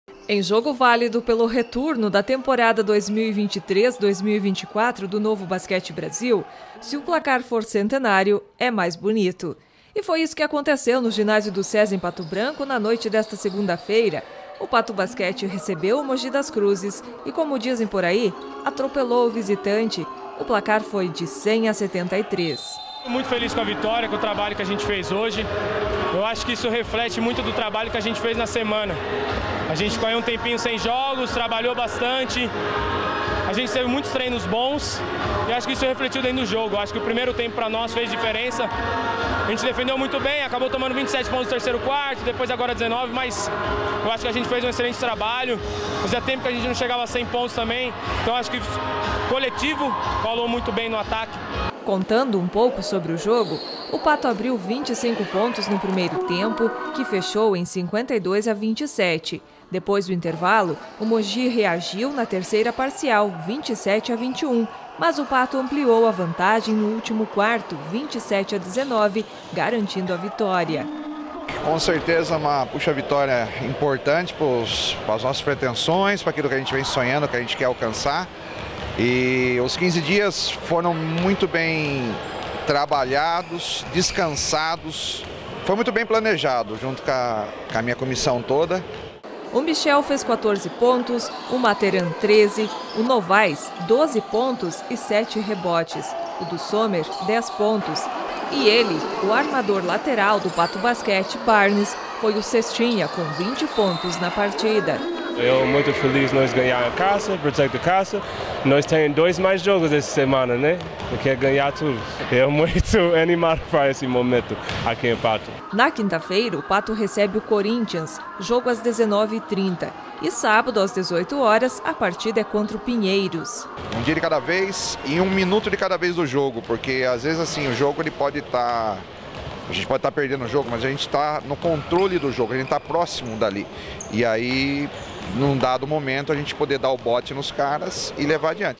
Pato Basquete vence Mogi das Cruzes, pelo placar de 100 a 73
Esportes: Com placar centenário, Pato Basquete vence o primeiro jogo em casa da semana. Partida foi contra o Mogi das Cruzes.